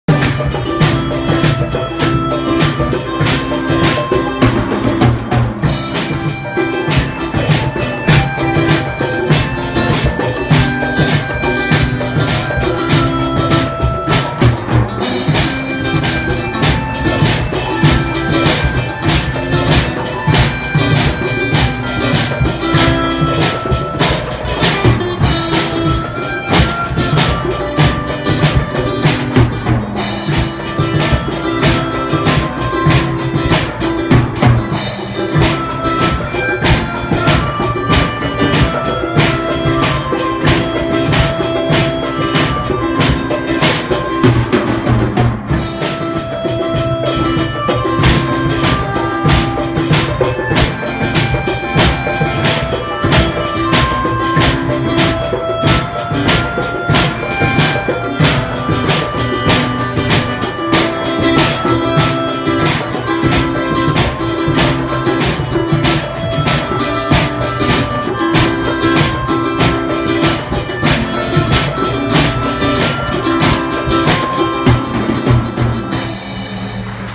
First performance with drums
A beginner with drums.. performance at MGM HIGHER SECONDARY SCHOOL morning assembly..